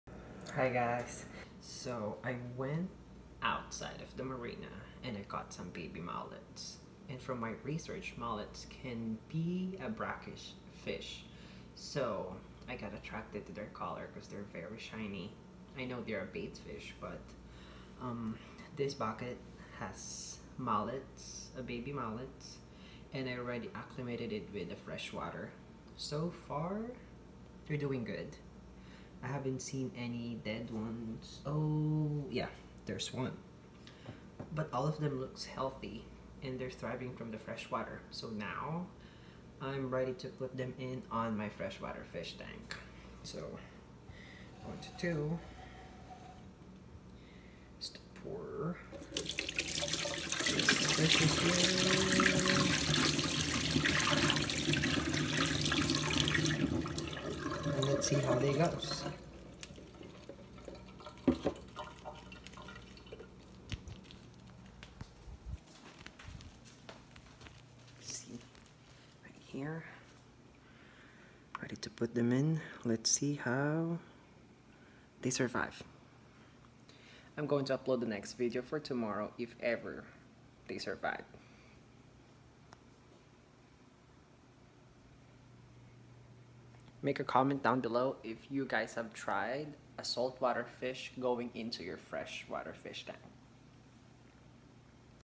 Salt water mullet fish in fish tank
After acclimating the salt water mullet for 2 hours i will try to put them in my 65 gallon fresh fish tank along with my guppies